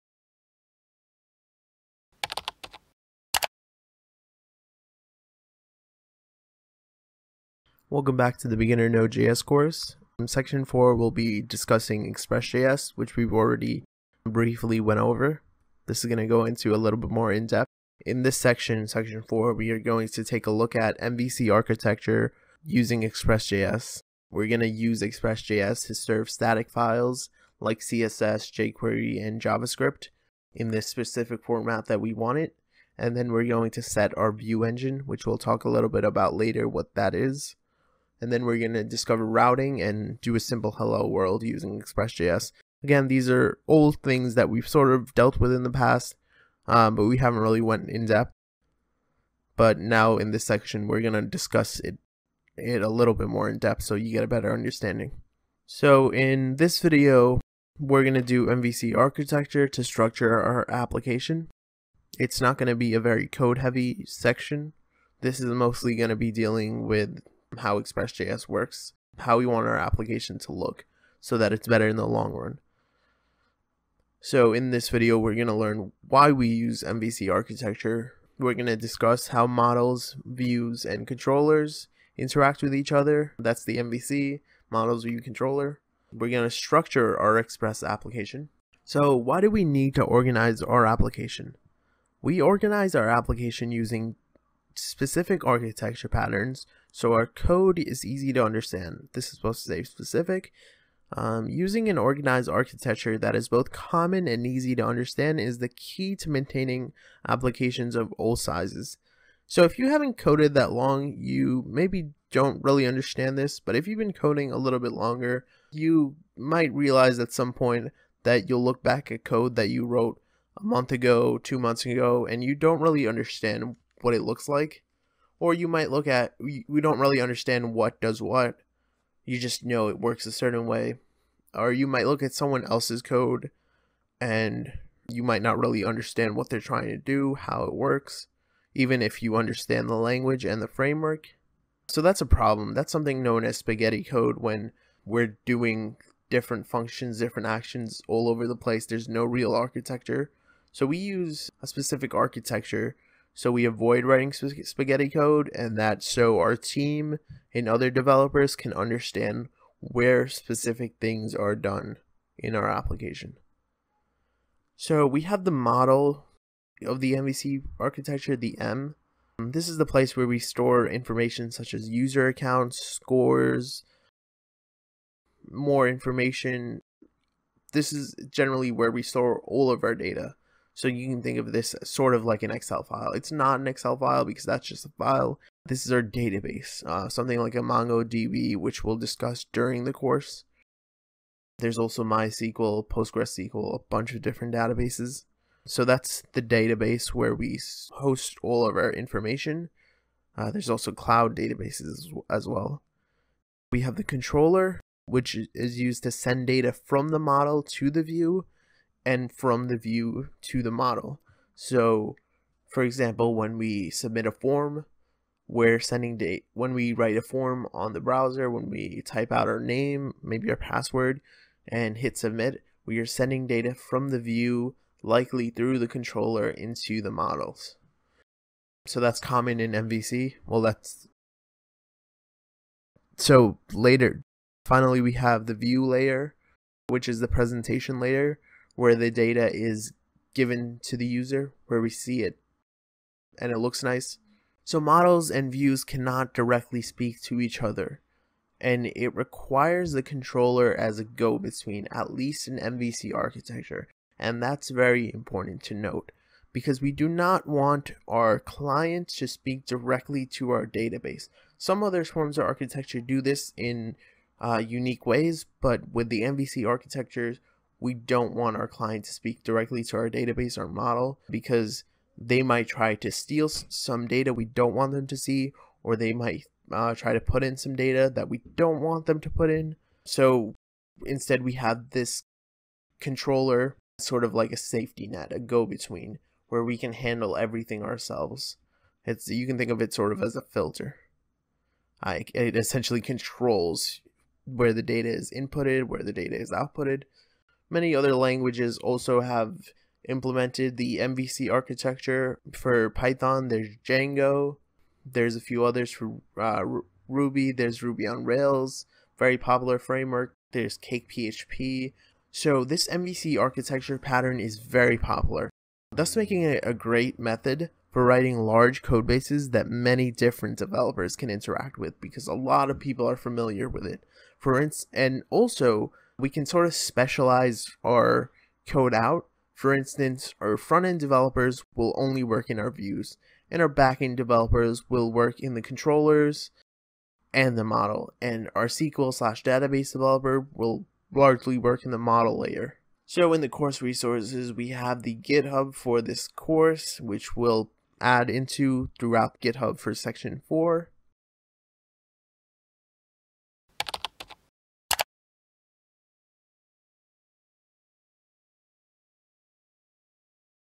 Server-side Programming with Node.js for Beginners : Using MVC to Structure Our App|packtpub.com
In this video, we will learn the use of MVC to structure our application.
• Learn why we use MVC architecture
• Discuss how models, views and controllers interact with each other
• Structure our express application